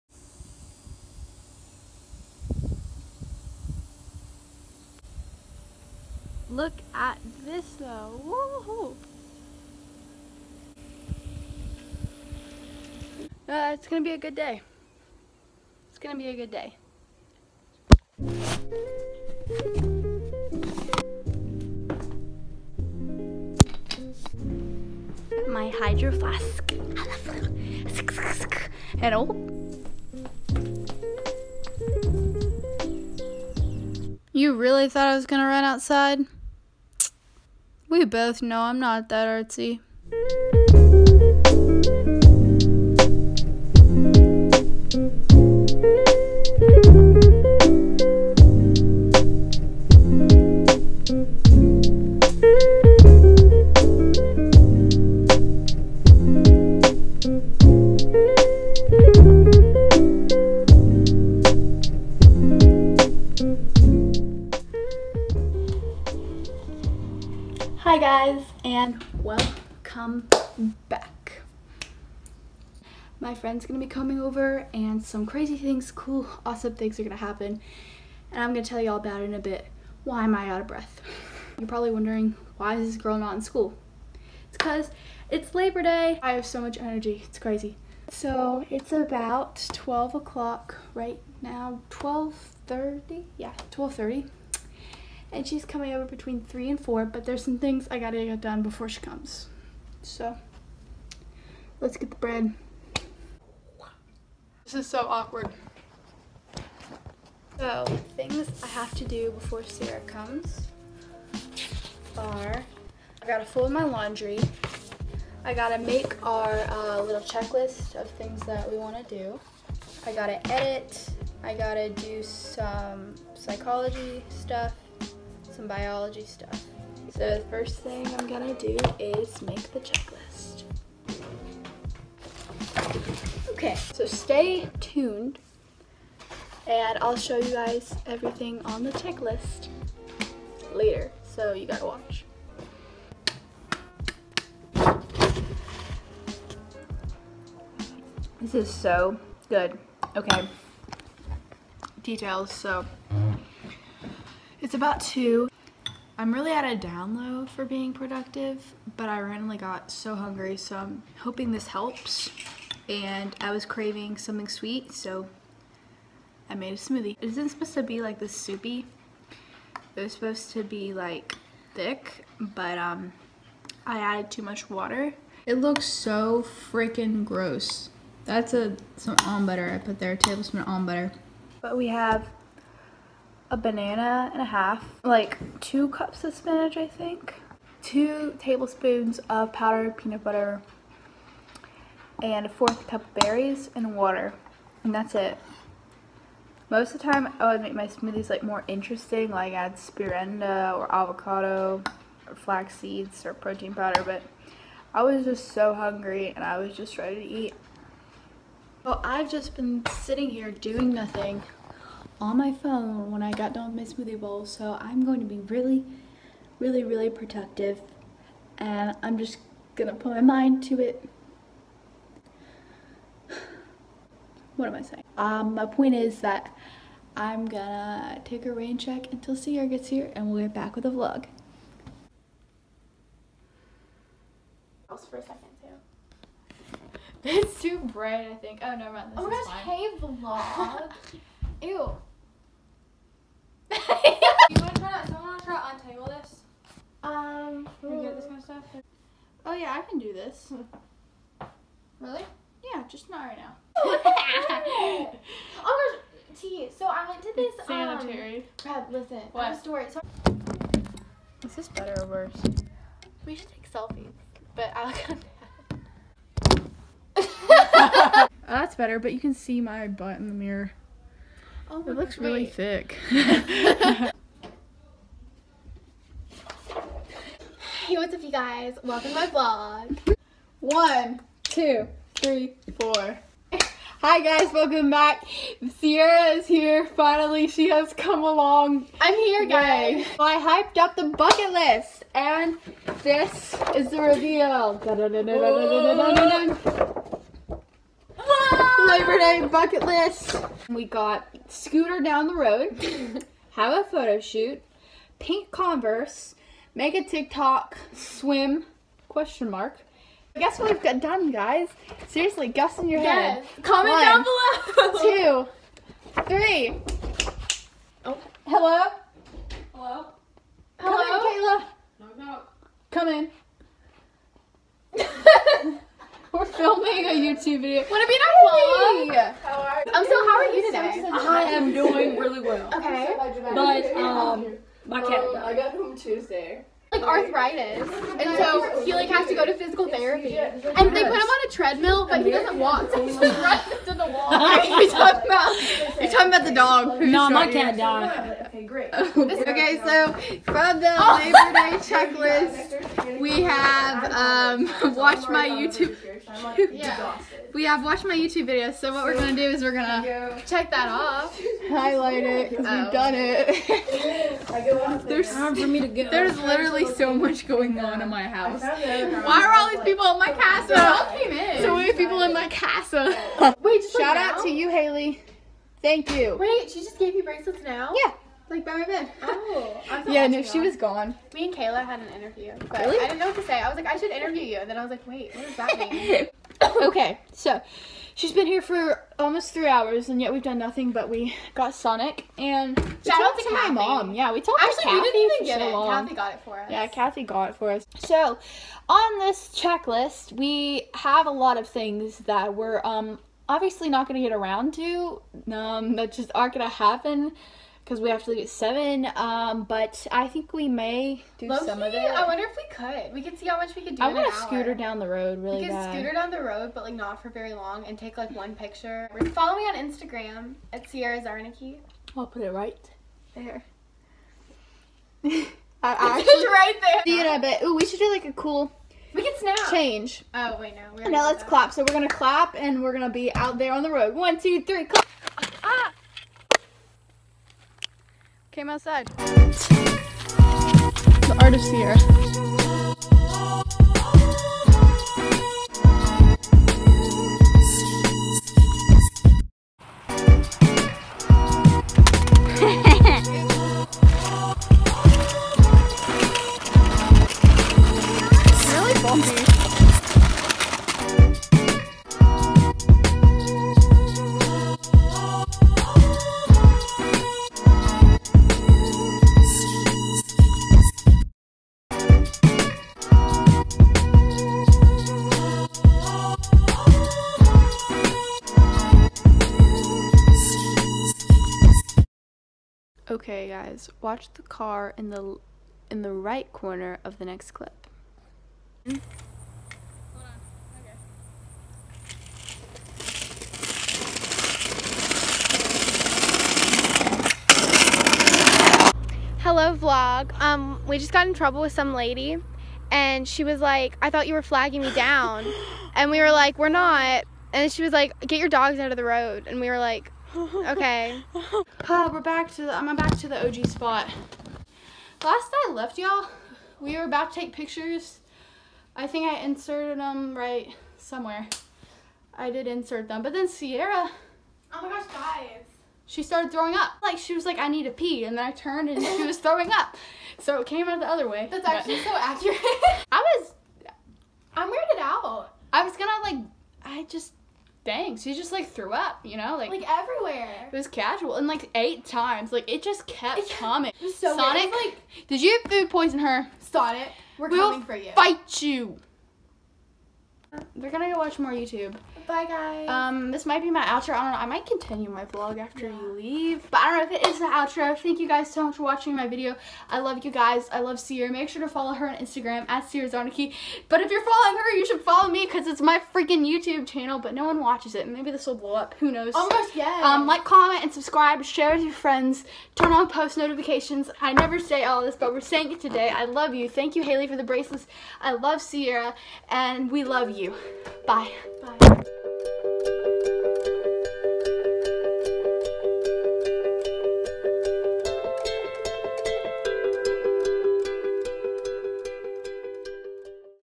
we tried to scooter on the road but got yelled at...LOL YOLO
fall of sophomore yr vlogs: episode one

make sure the quality is on 1080p!

xoxo
reb